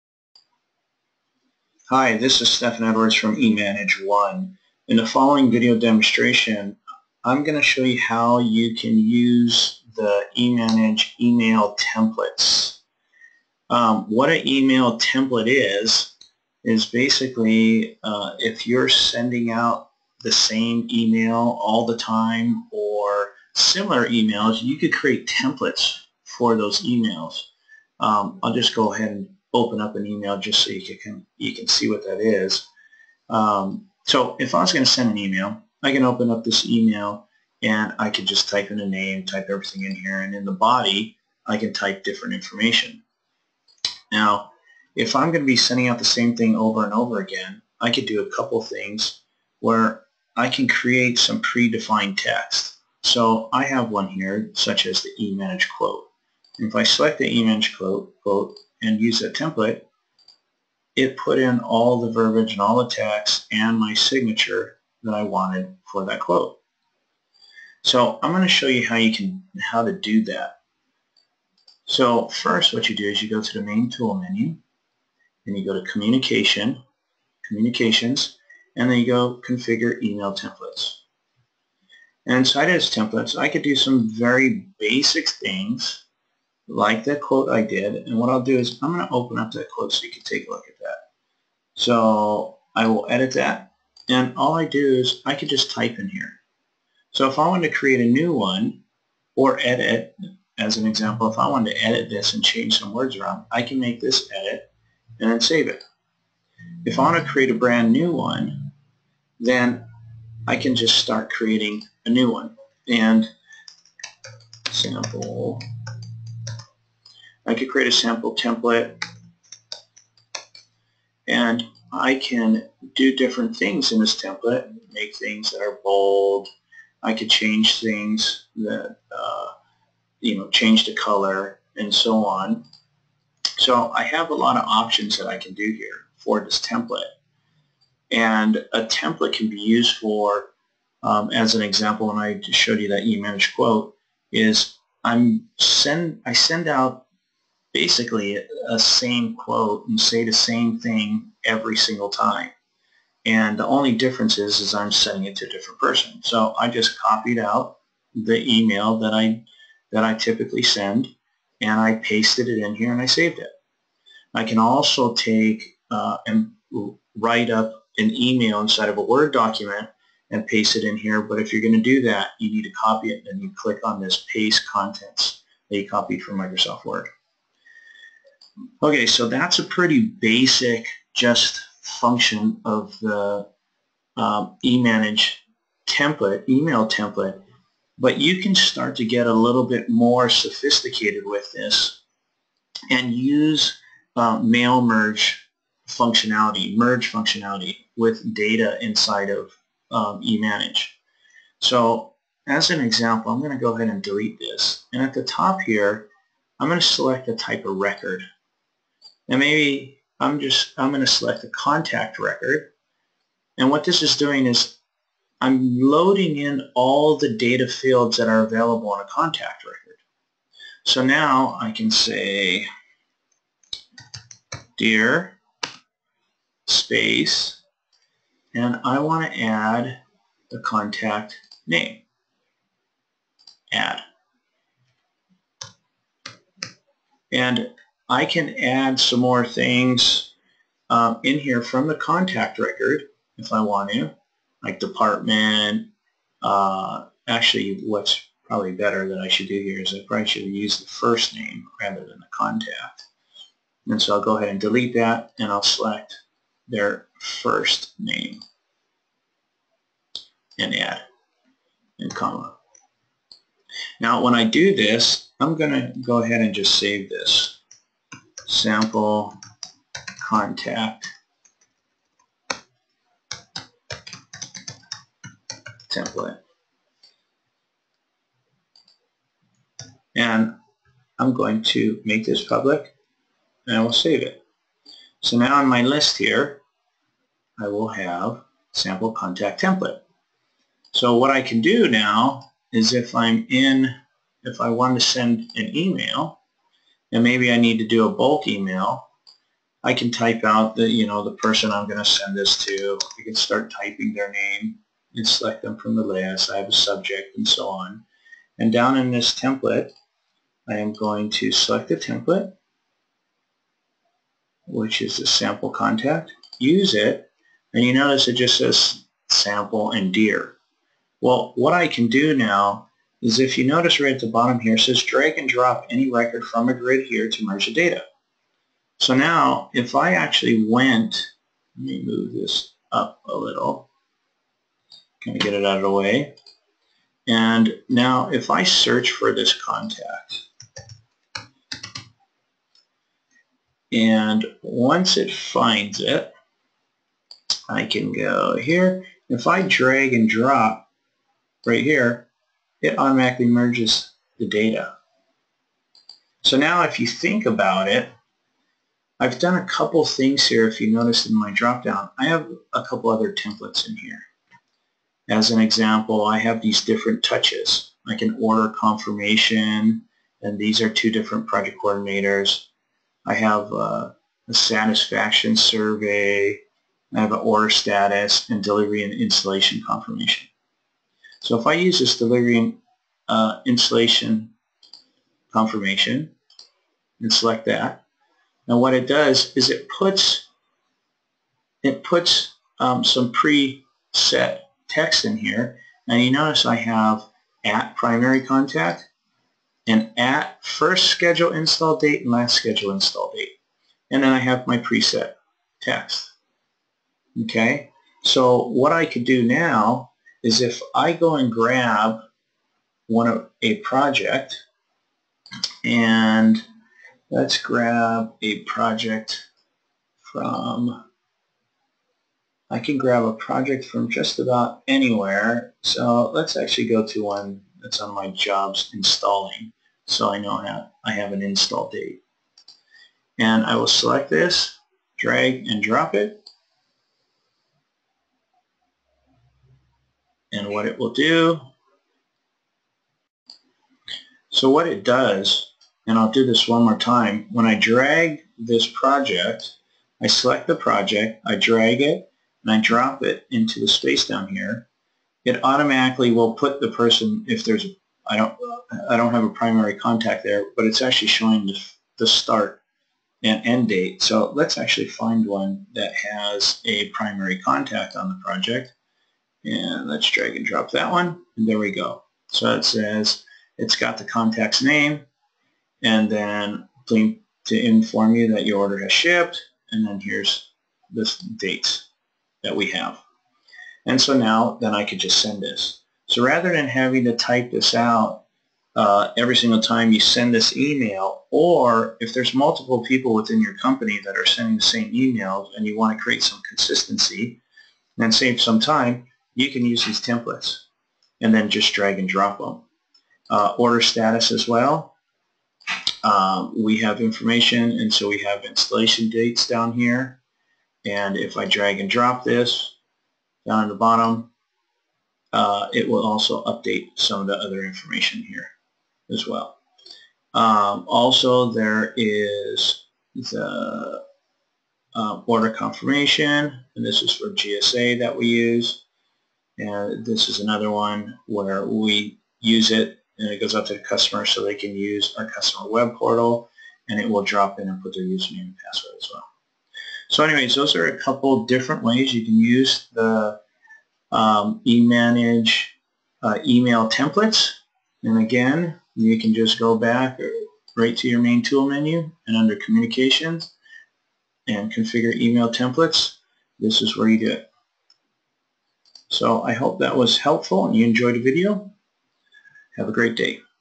Using Email Templates
Create custom email templates to save your team time, create standards within your organization and email blasts to customers for marketing.